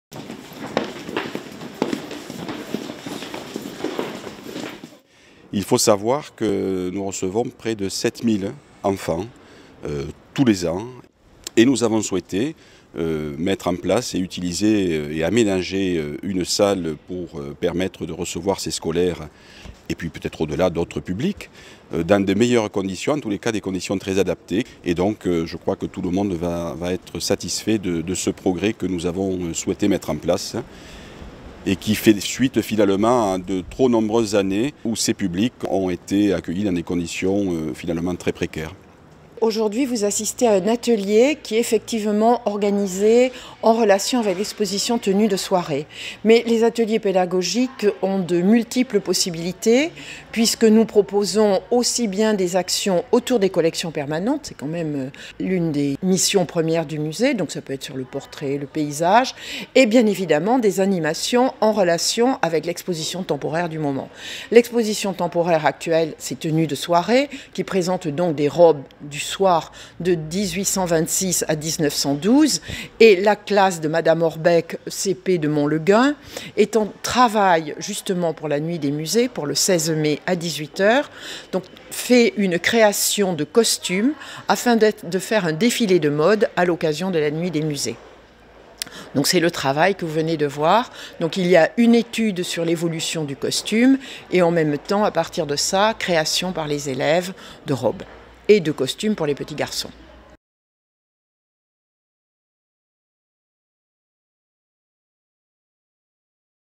Ouverture du local pédagogique au Musée des Beaux-Arts
Le Musée des beaux-arts de Carcassonne accueille, chaque année, entre 5000 et 7000 élèves, de la maternelle à l’enseignement supérieur, provenant d’établissements scolaires de la Ville, mais également du département de l’Aude, voire de la France entière.
Ces ateliers pédagogiques sont ouverts à tous les établissements scolaires, sur simple rendez-vous auprès de l’accueil du musée des beaux-arts. La participation est entièrement gratuite et le matériel est fourni gracieusement par le Musée de la Ville.
Pour accueillir ces artistes en herbe, un nouveau local pédagogique a été aménagé dans l’enceinte même du Musée. Pour la première fois, les élèves d’une classe de CP du groupe scolaire de Montlegun participent, à une animation autour de l’exposition temporaire « Tenues de Soirée ».